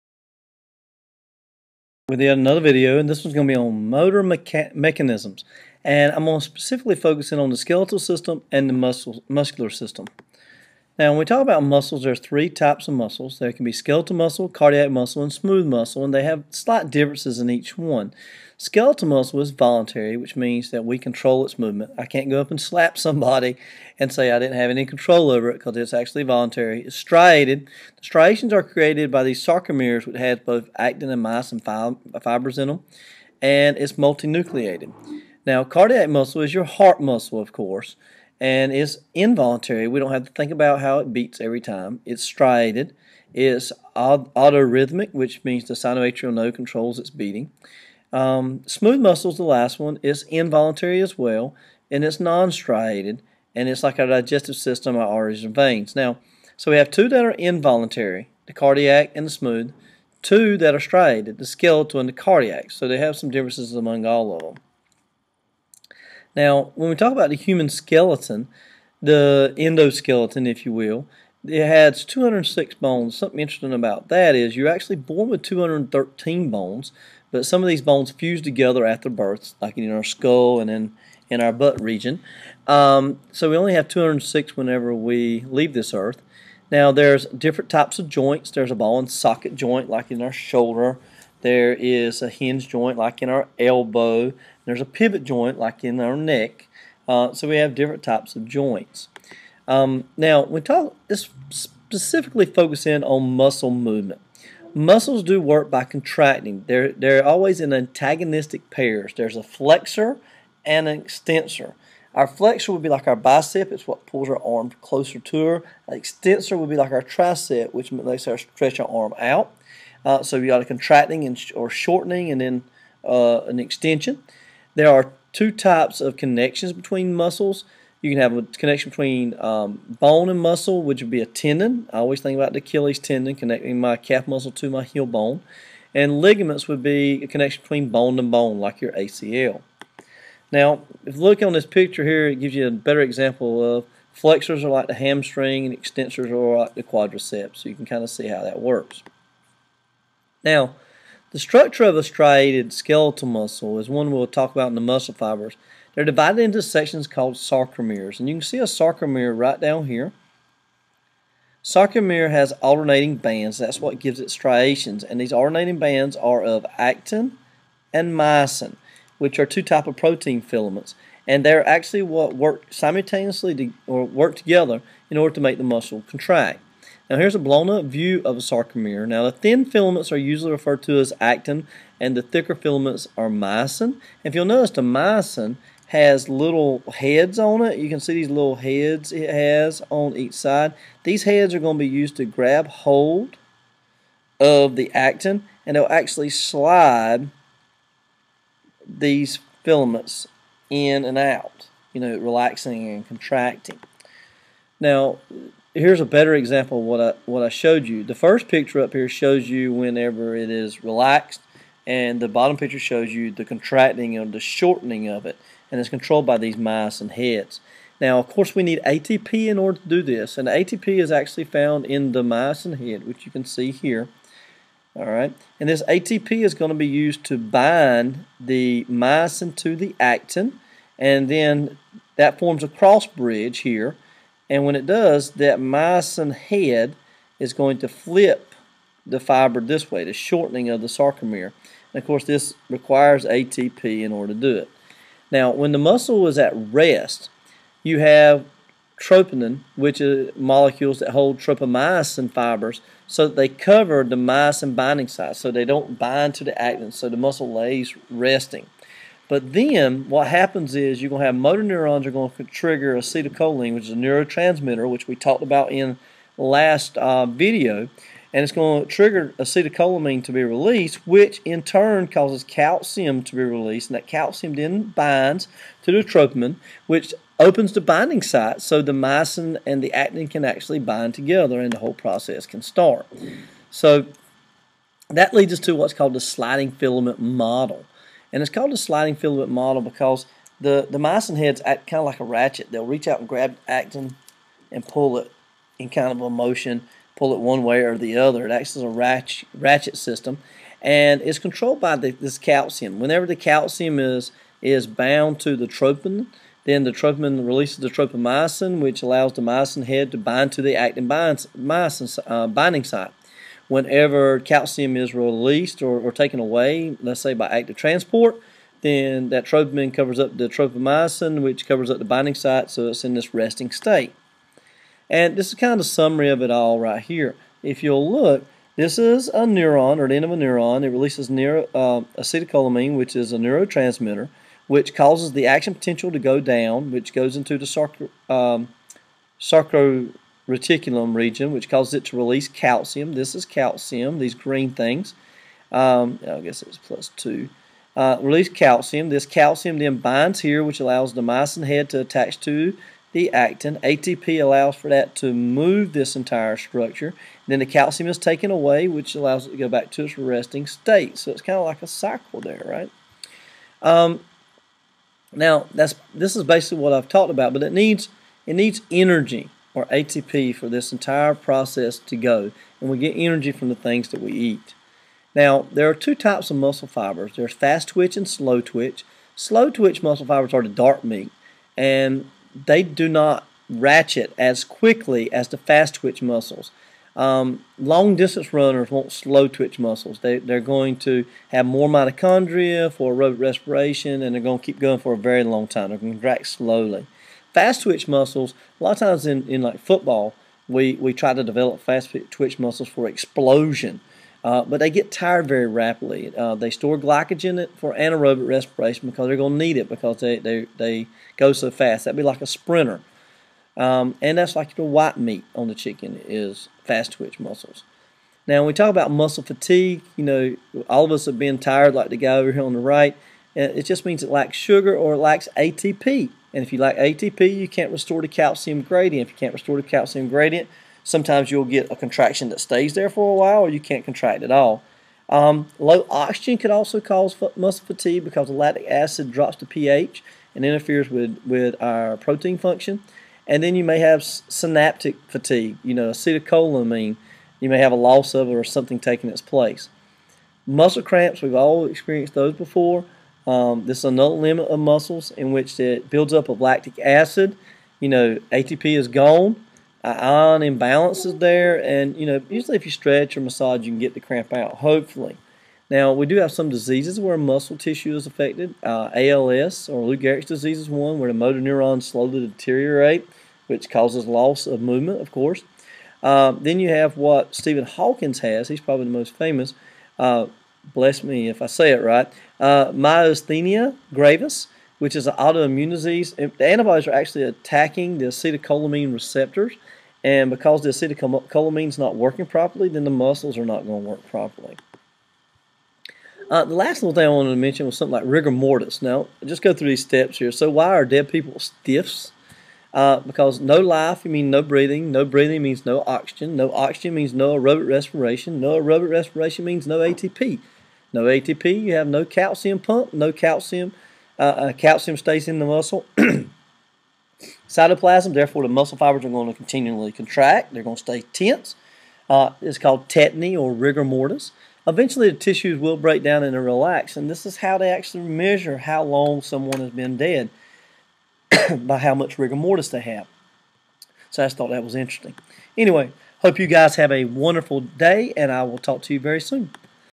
Motor Mechanisms
Description of the muscles and skeleton.  Focus on how a muscle works.